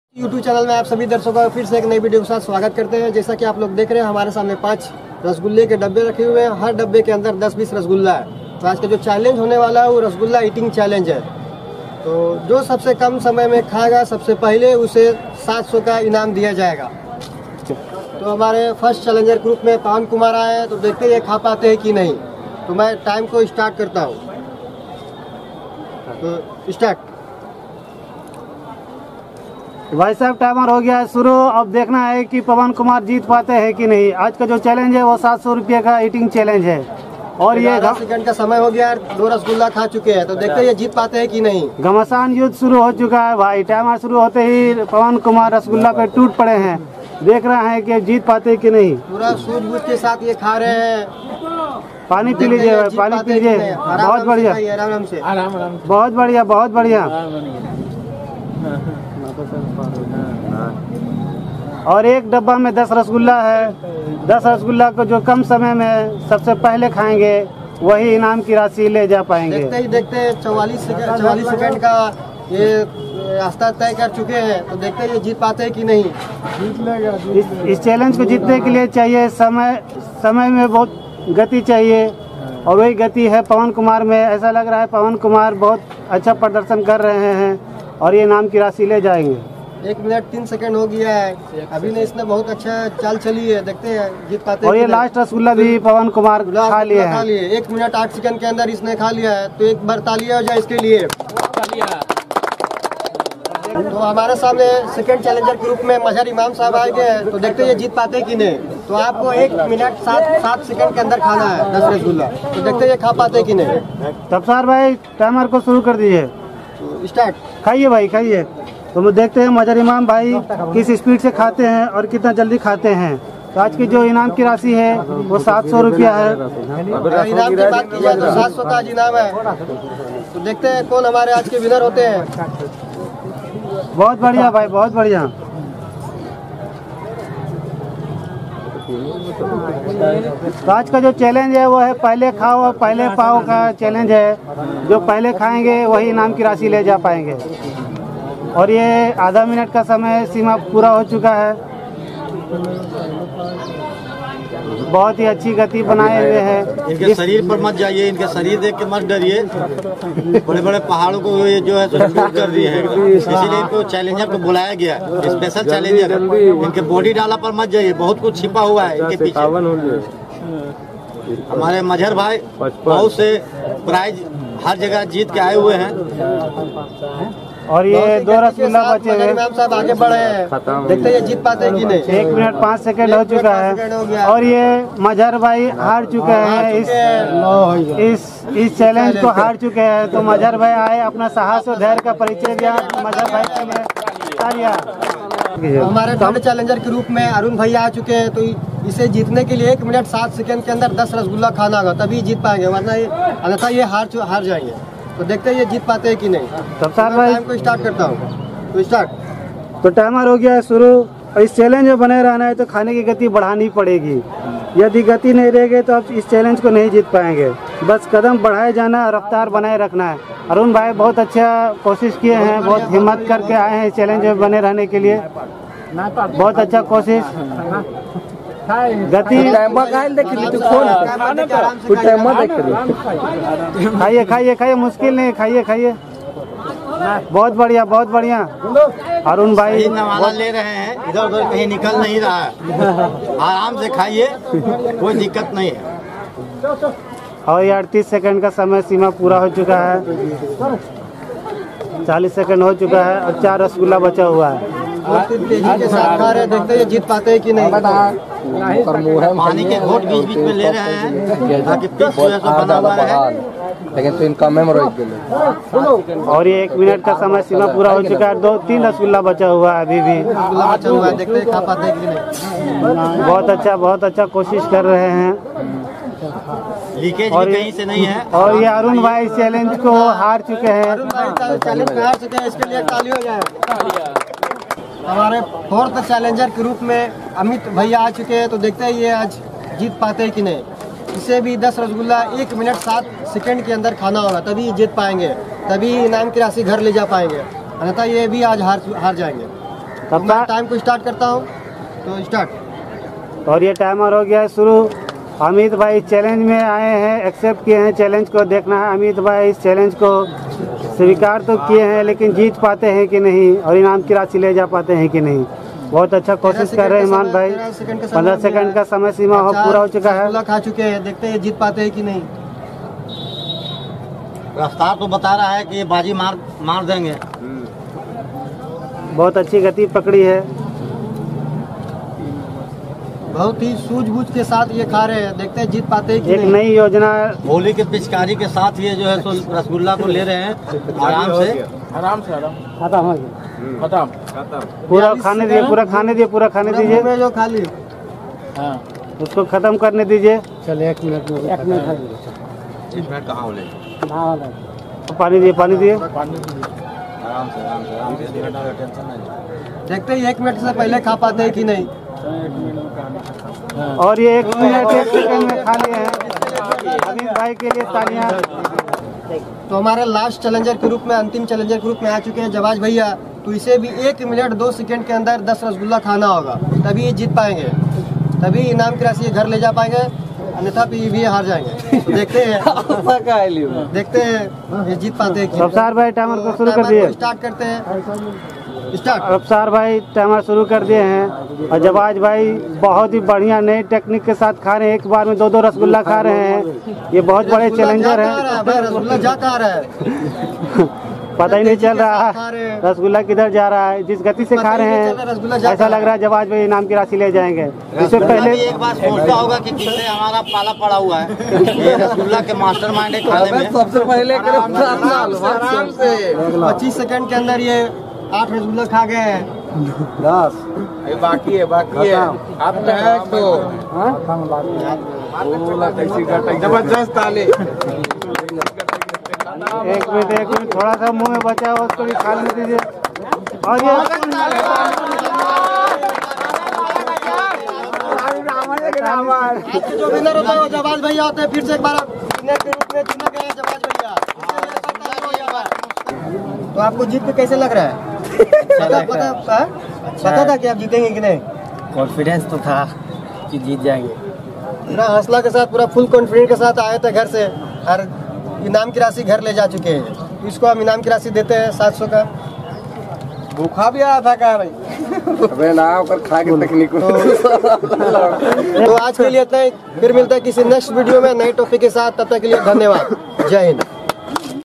सबसे पहले 10 रसगुल्ला खाओ 700 ले जाओ 🤑 || Eating Rasgulla sweets Challenge.
This video is all about eating rasgulla sweets challenge. whoever will eat first Will win.